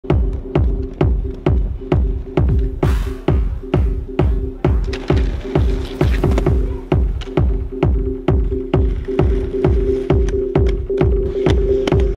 Skating everything in sight
Rollerblading with Muzzle means you skate everything in sight